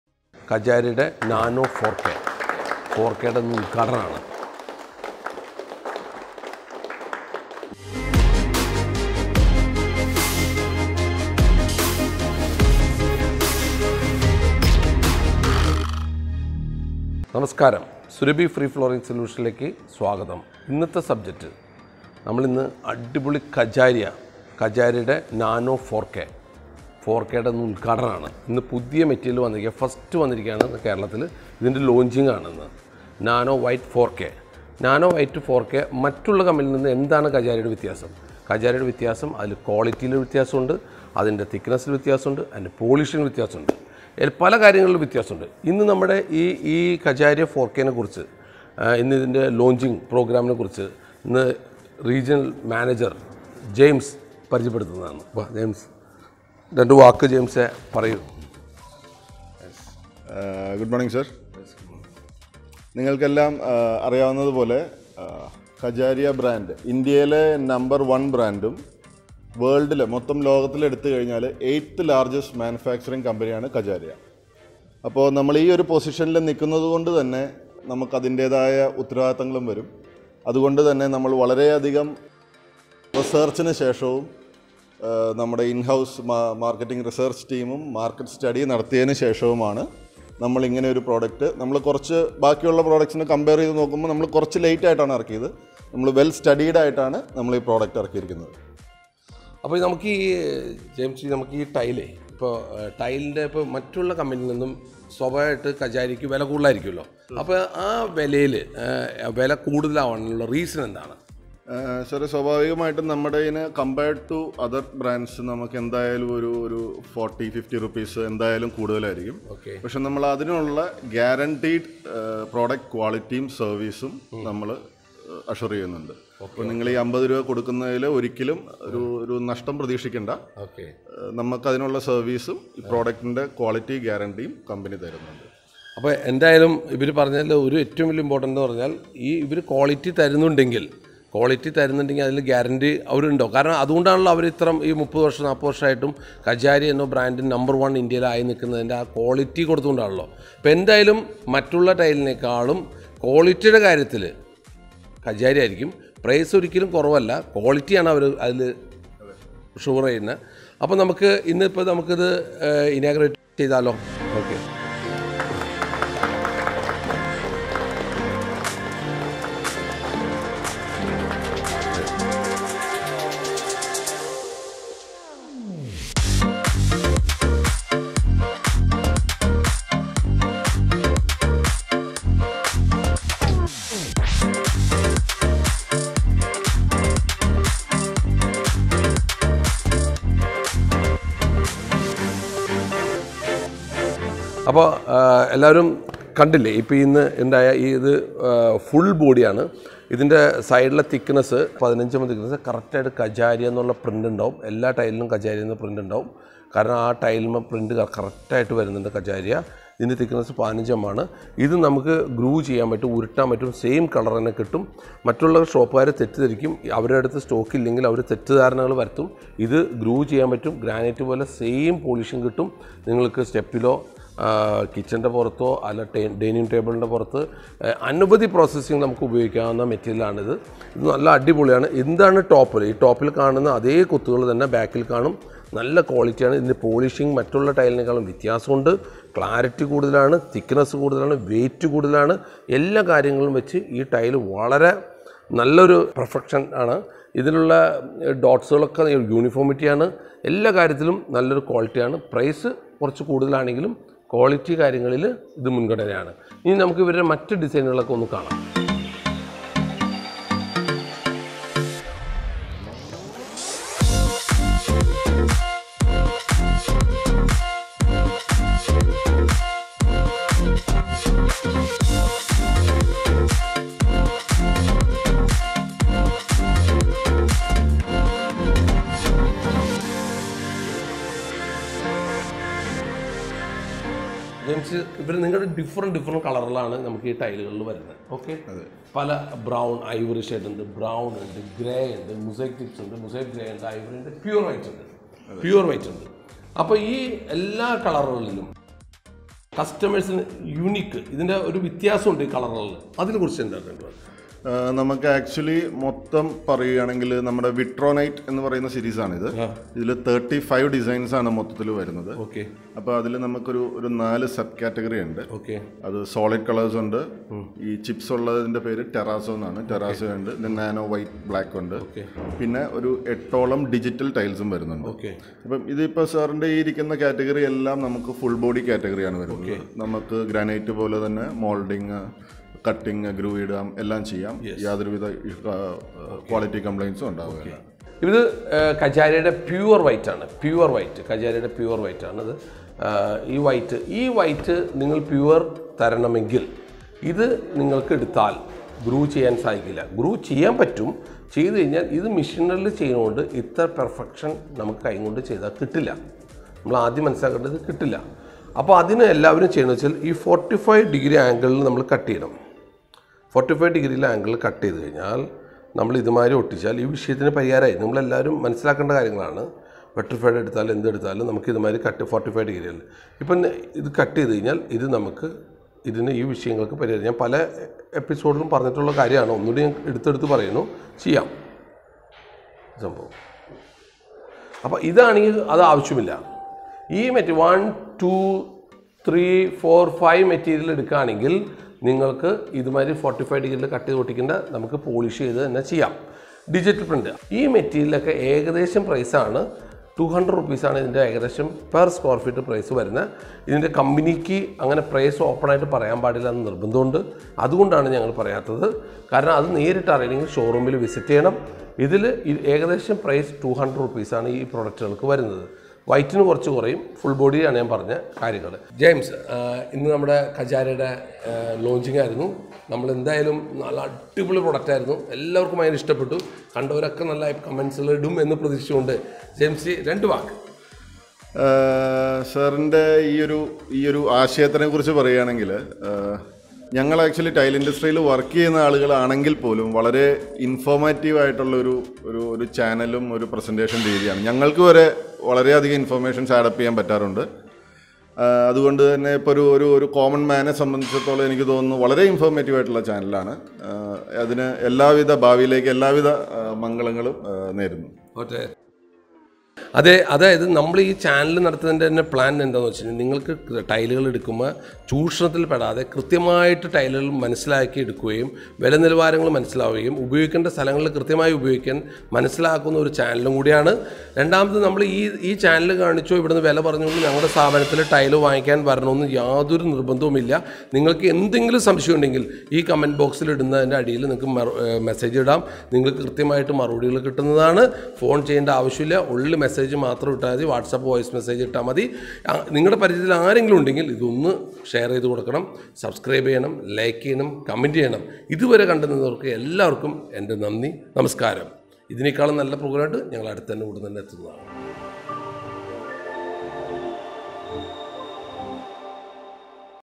Kajaria Nano White 4K കണ്ടിട്ടുണ്ടോ? | New arrival of Nano White 4K | Surabhi Innovation.
വീടു പണിയുടെ അവസാന ഘട്ടത്തിൽ ചെന്നെത്തി നിൽക്കുമ്പോൾ  വ്യത്യസ്തതയുള്ള  ഒരു flooring material വേണമെന്ന് മനസിൽ ആഗ്രഹിക്കാത്തവരായി ആരുണ്ട്?

കാലാകാലങ്ങളായി എല്ലാവരും വിരിക്കുന്ന ടൈലുകളിൽ നിന്നും തികച്ചും വ്യത്യസ്തമായ concept കൊണ്ടുവരുന്ന ഒരു പുതിയ ഐറ്റം - Kajaria Nano White 4k ഒന്നു കണ്ടു നോക്കിയാലോ? സത്യത്തിൽ ഇത് floor ൽ വിരിച്ചു കണ്ടാൽ ഞെട്ടും!!

കൂടാതെ ഞങ്ങൾ പരിചയപ്പെടുത്തുന്നു എല്ലാവിധ flooring material കളുടെയും വില, ഗുണനിലവാരം പരിശോധിക്കാം

വിലകുറവും ക്വാളിറ്റിയും സൊല്യൂഷനും

Surabhi Innovation
Mc Road, Angamaly
Only Whatsapp voice chat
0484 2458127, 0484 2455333
Open Time 10:00​​​​​​Am - 06:00​​​​​​​​​​Pm

AND